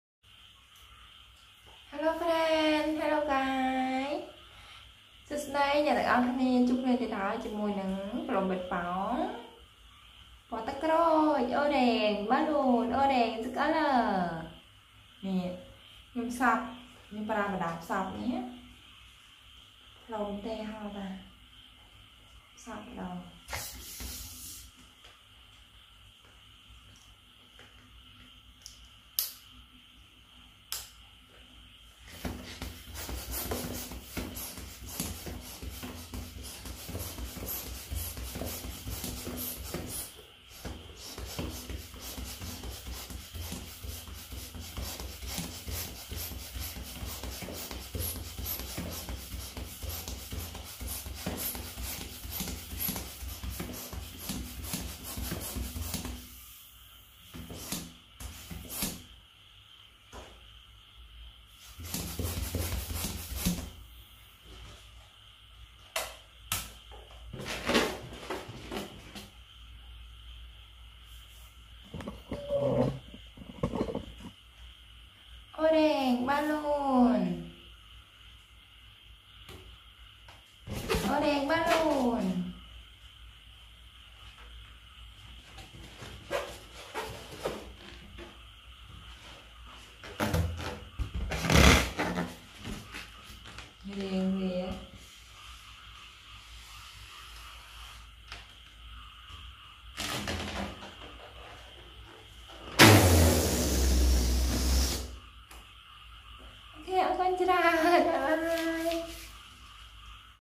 outdoor fun with pumping balloons learn colors
Welcome to the Oun neth Channel..!Ounneth
oun neth channel shows you how to learn colors by blowing balloons.